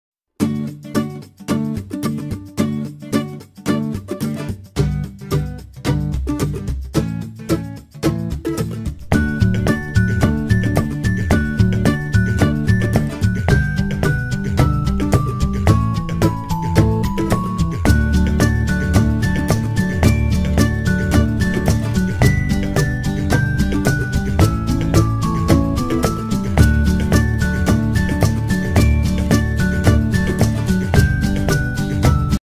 El acuario d3 mi niña / My daughter s tank (video 4k hd)
Acuario comunitario plantado low tech
Video HD 4K